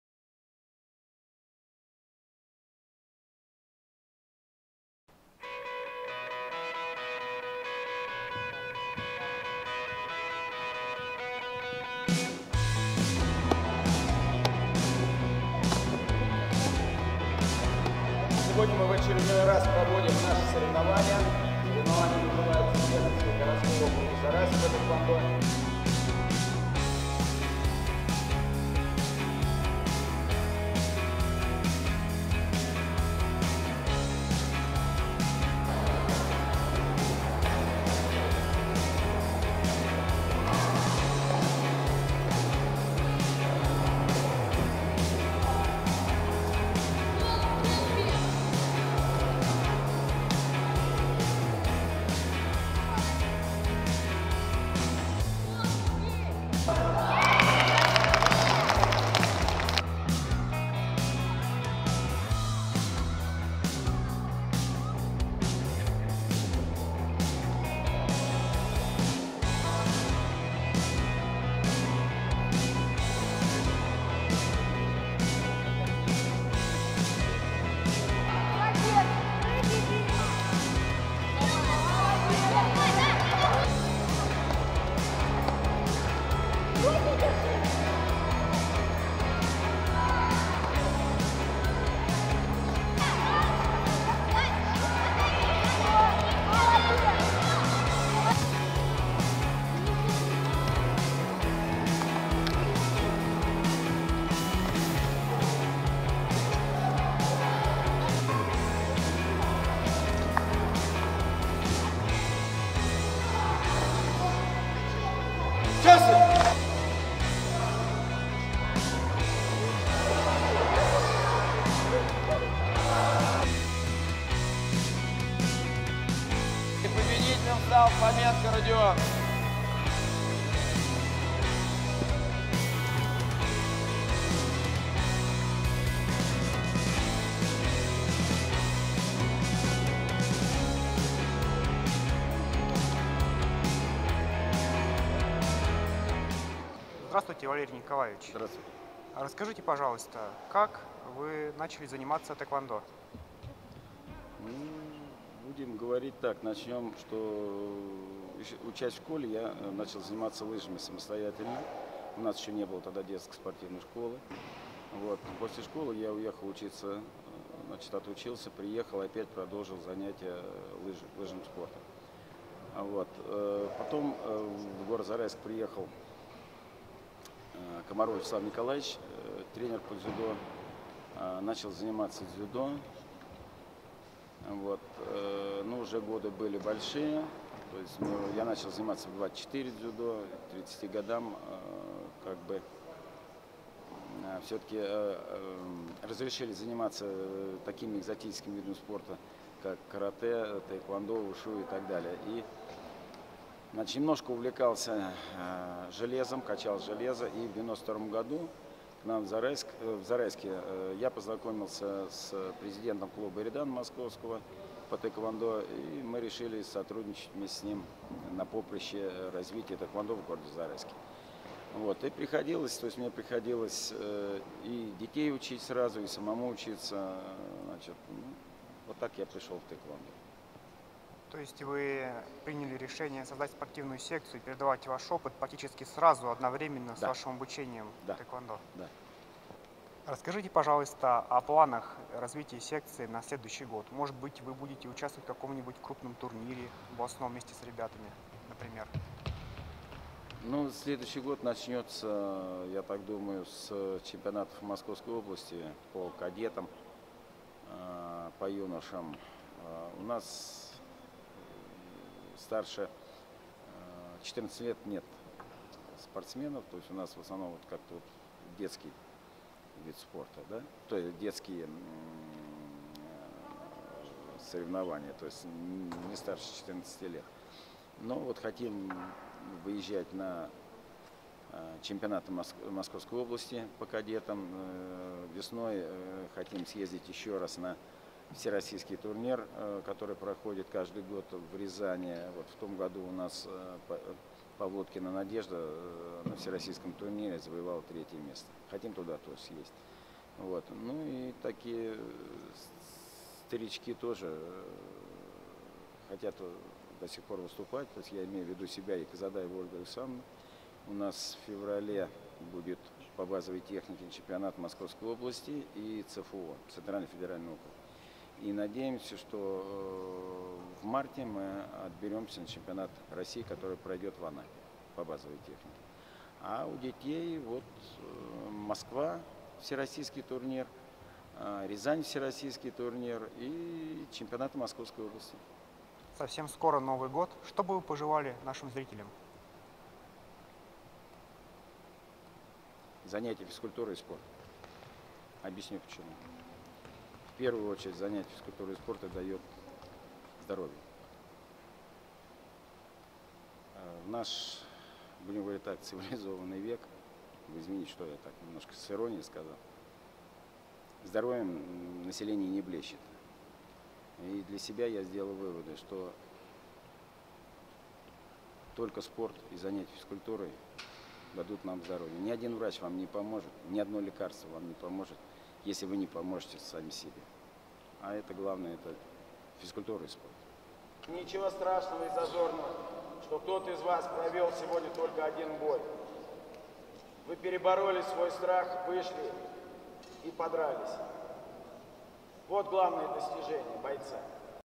Первенство по тхэквондо в Зарайске
24 декабря во Дворце спорта »Зарайск» прошло первенство г.о. Зарайск по тхеквондо (ВТФ) керуги. В боях приняли участие 77 спортсменов в 24 категориях.

Победители и призеры были награждены медалями и кубками. Также были разыграны 4 кубка за волю к победе и 4 кубка за лучшую технику.

Родион Фоменко, признанный лучшим бойцом соревнований, награжден японским мечом-катана.

Автор - Евгений Мазепов